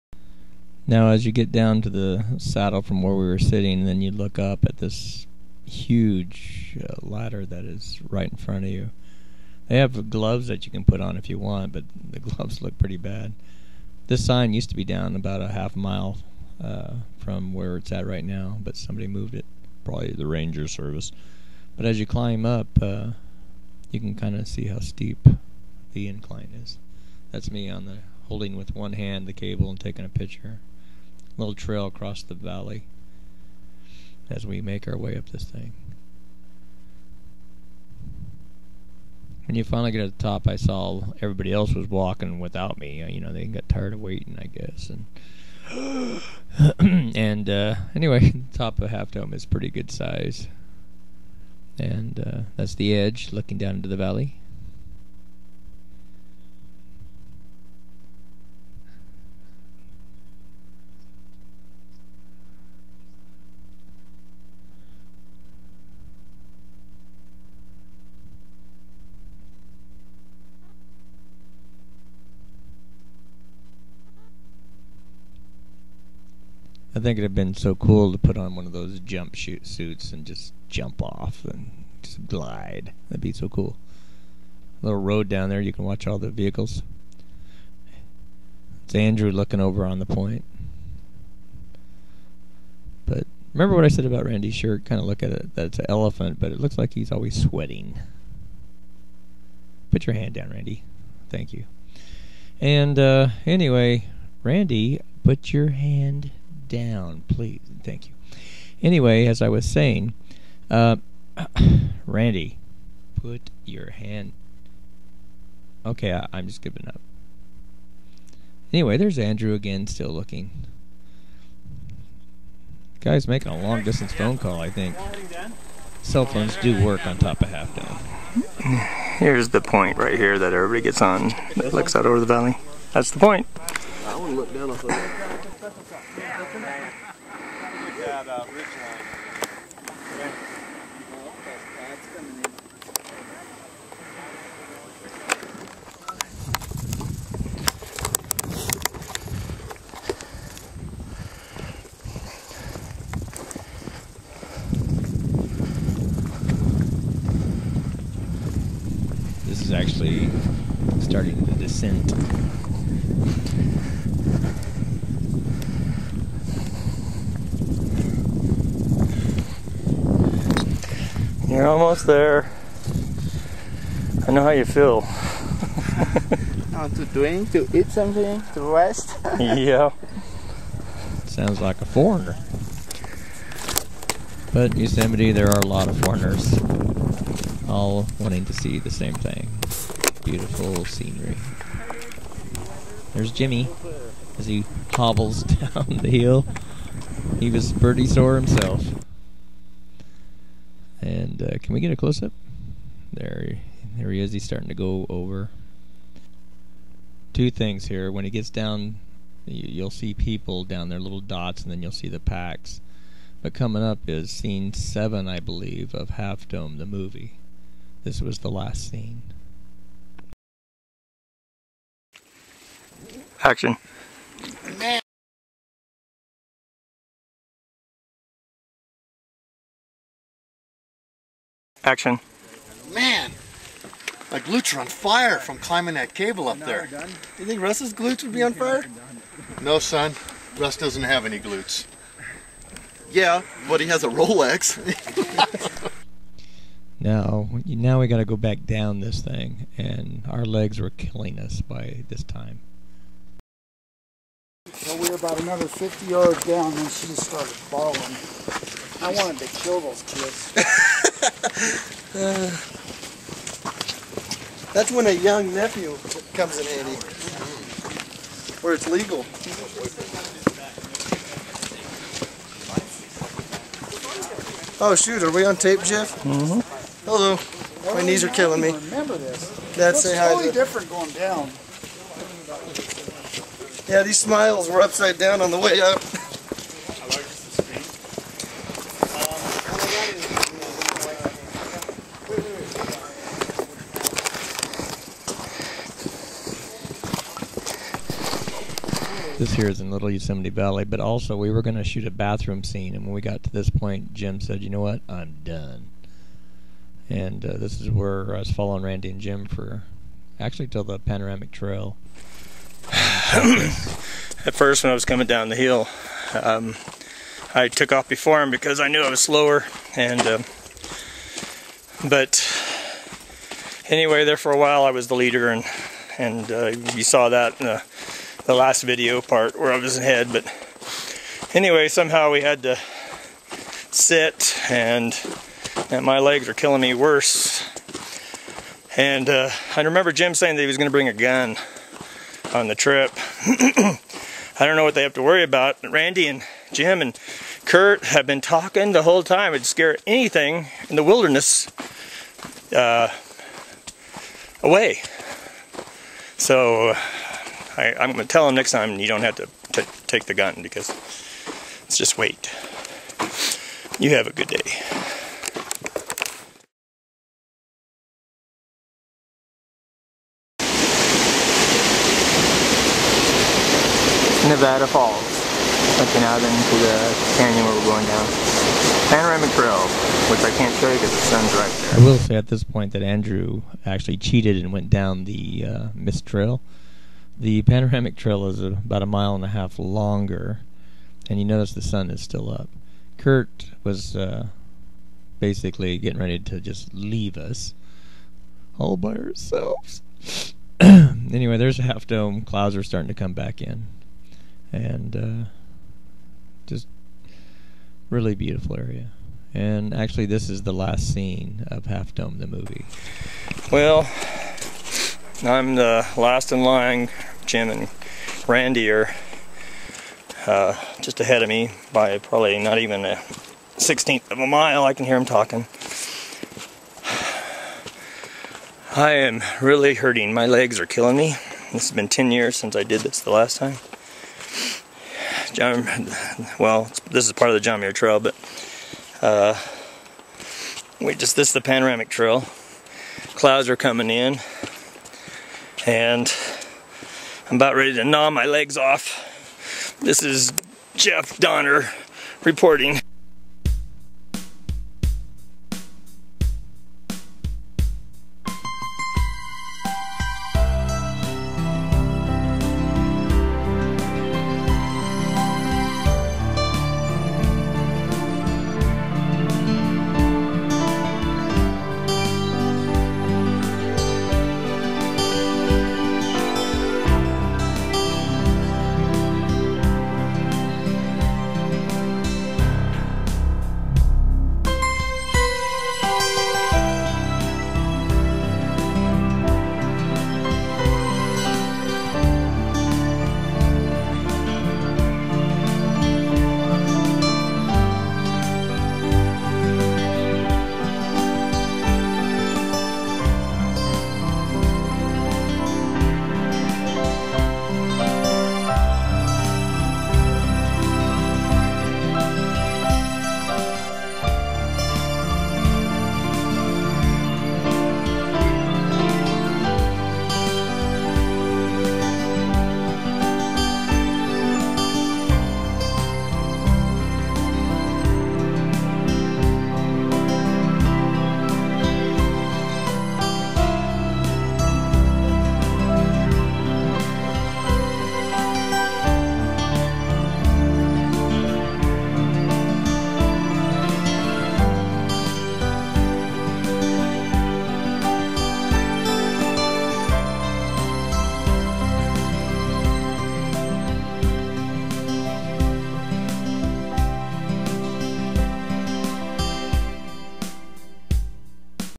HD the movie part 6
Half Dome the movie part 6. We climbed Half Dome in Yosemite, Ca. last year, 2008. This is the expanded version of the 4 min. lampoon of Uncle Russ,(Half Dome The Movie) who did not want to go last year, and this year (yes we are doing it again) on Sept. 26th. and Russ is going this year.